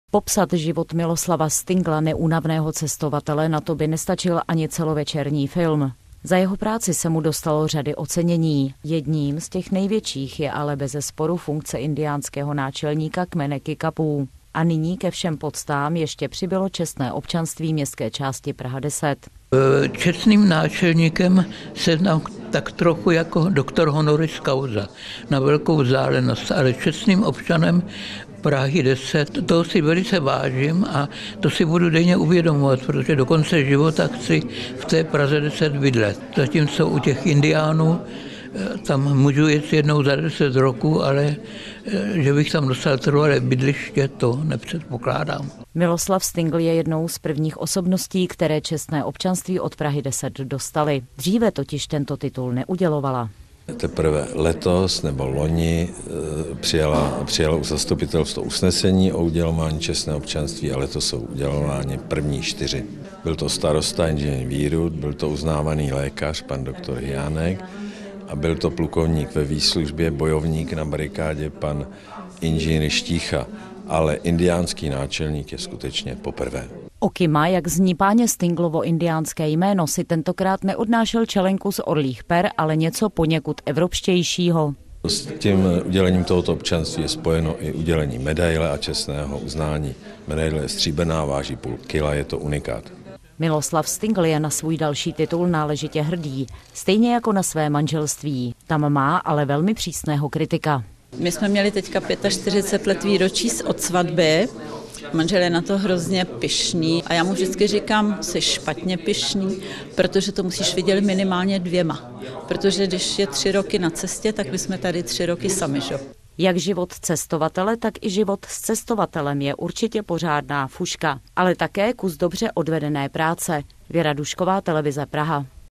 Desítka udělila čestné občanství cestovateli
Praha 10 udělila čestné občanství cestovateli a indiánskému náčelníkovi. Pan Miloslav Stingl dostal čestné uznání a stříbrnou medaili.